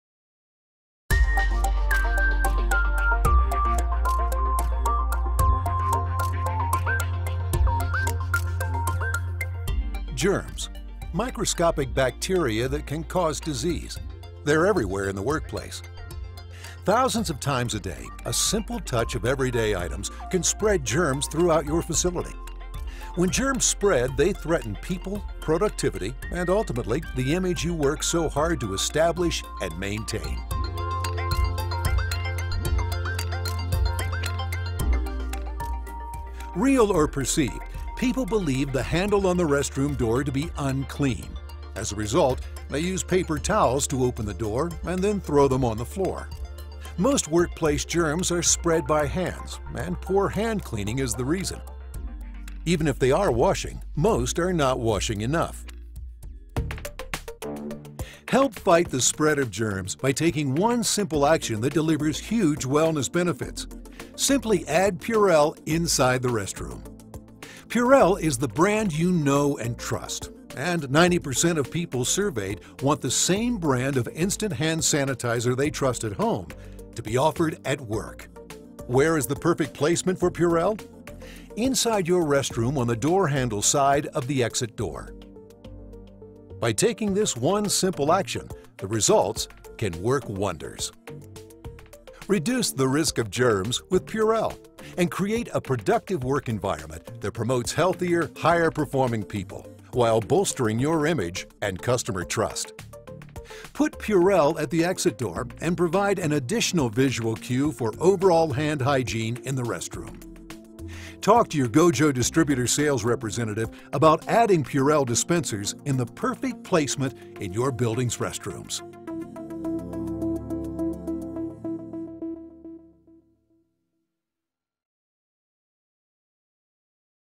GoJo - PURELL® Perfect Placement for Restrooms
Although this video is obviously american, it gives a very good insight into the use of hand sanitising units in the washroom. Also lots of very interesting statistics - for example 1 out of 4 people don't wash their hands after using the toilet (or didn't in 2007!!)